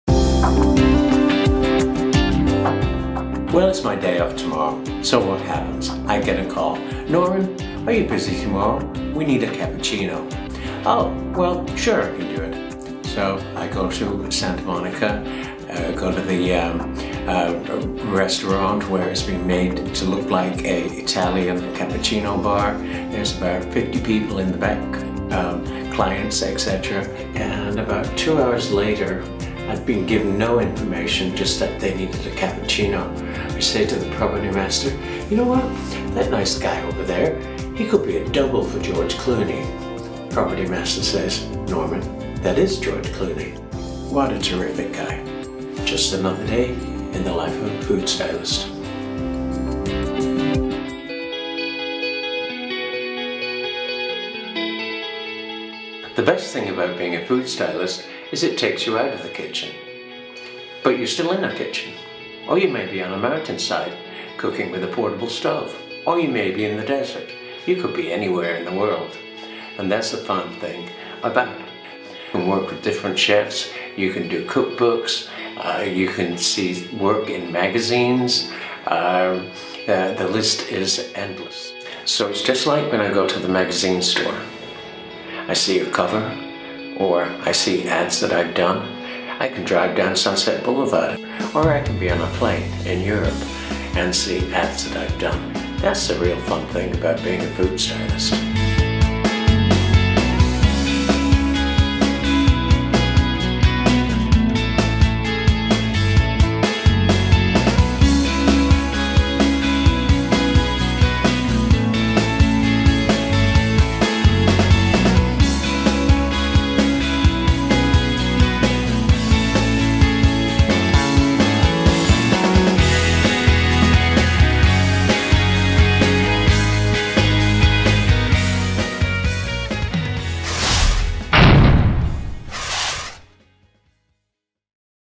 Los Angeles Food Stylist Norman Stewart
This is an Interview of Los Angeles food stylist Norman Stewart. He styles 8 Bon Appétit magazine covers a year and has worked with many celebrities such as George Clooney and Britney Spears. This video was directed by Justin Stovall and edited by Justin Stovall and Dennis Davis. The footage was shot with a Canon 5D Mark II video camera and edited in Adobe Premiere CS5.